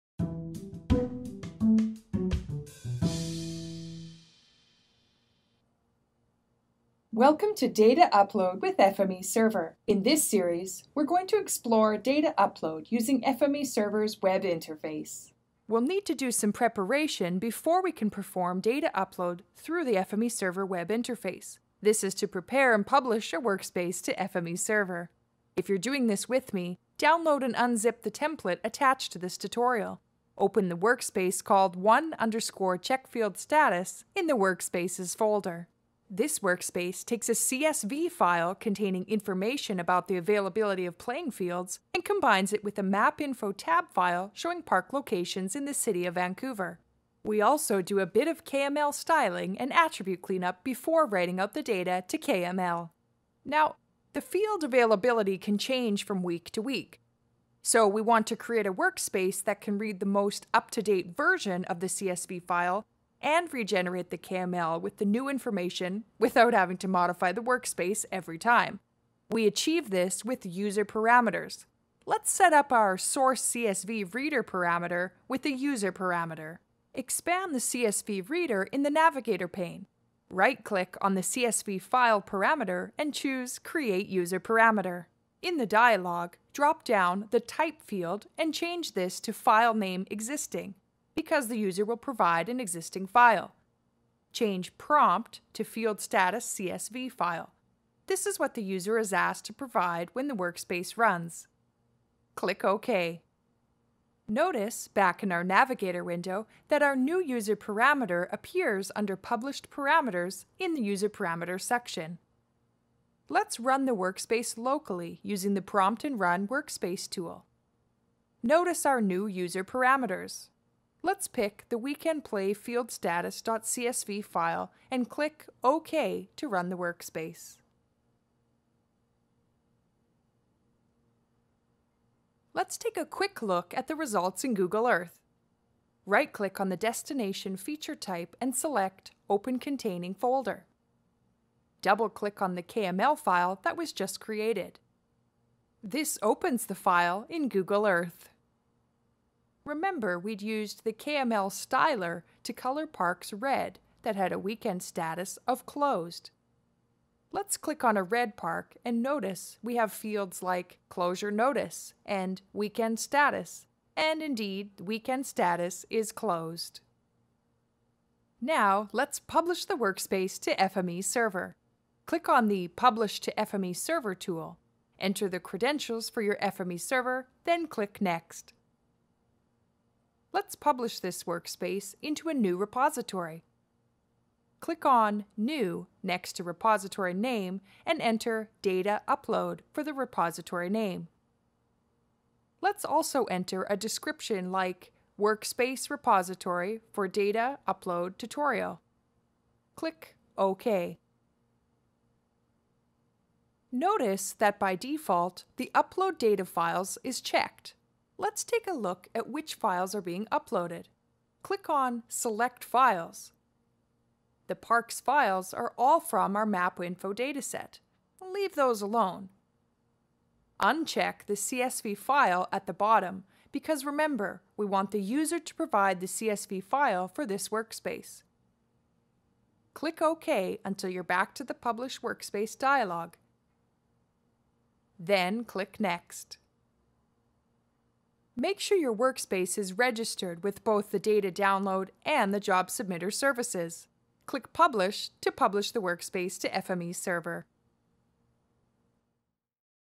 Data Upload with FME Server: Options from the FME Server Web Interface
Explore the options available in FME Server to make datasets available for use workspaces published to FME Server.  Here we will choose to use the options built into the default FME Server web interface.  We'll start by preparing a workspace to publish to FME Server.